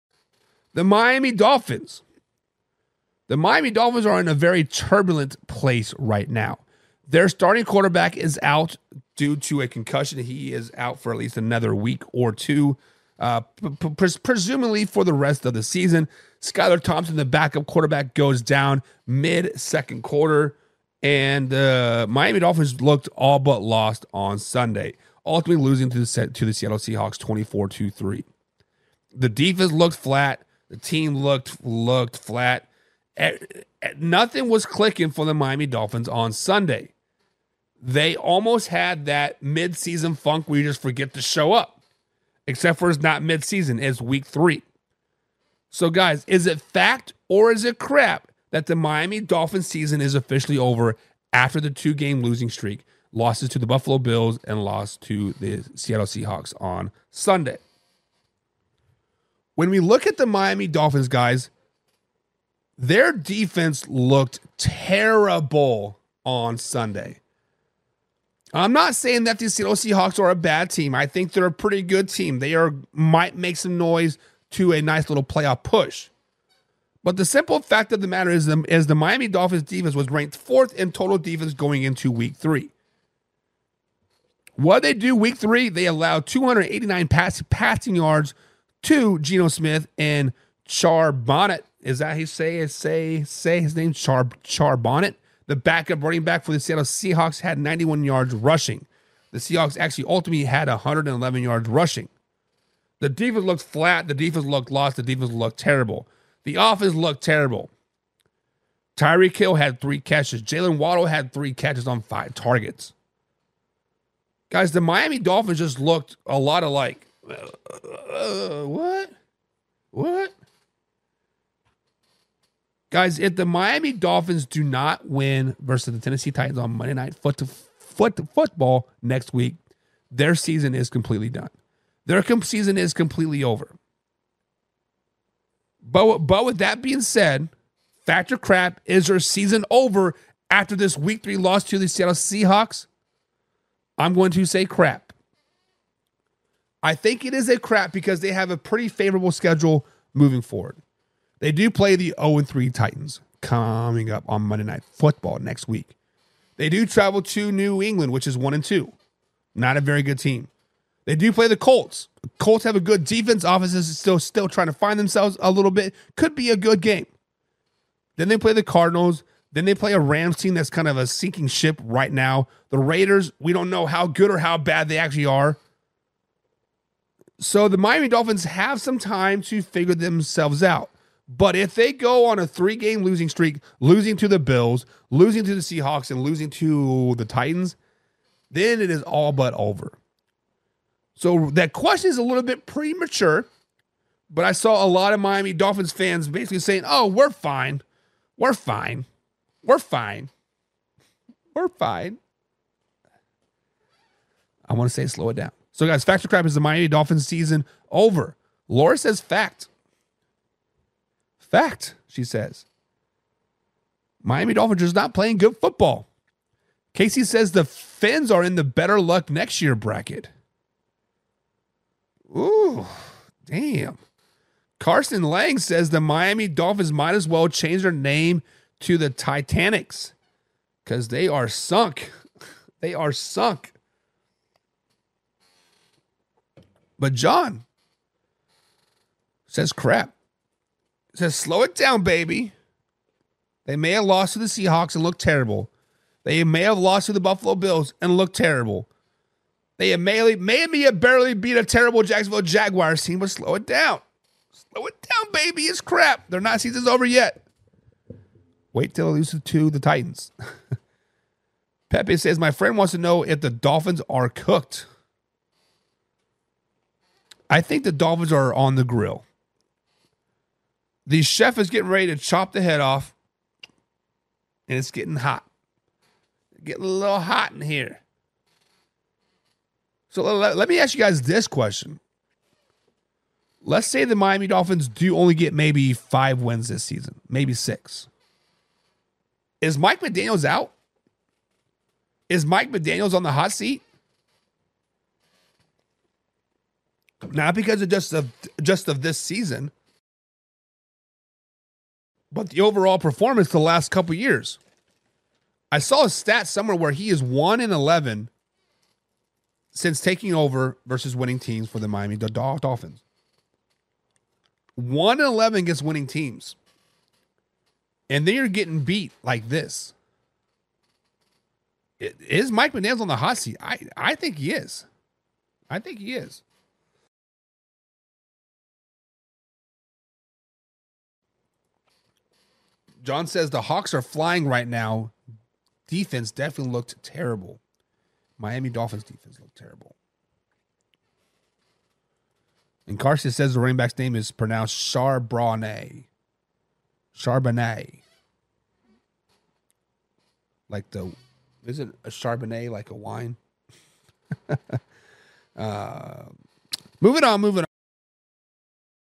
Dolphins season is over?
Dolphins fall to the Seahawks on Sunday, Another QB goes down, is their season over?
🚨🚨Join us live every Monday - Friday at 2:30 pm est🚨🚨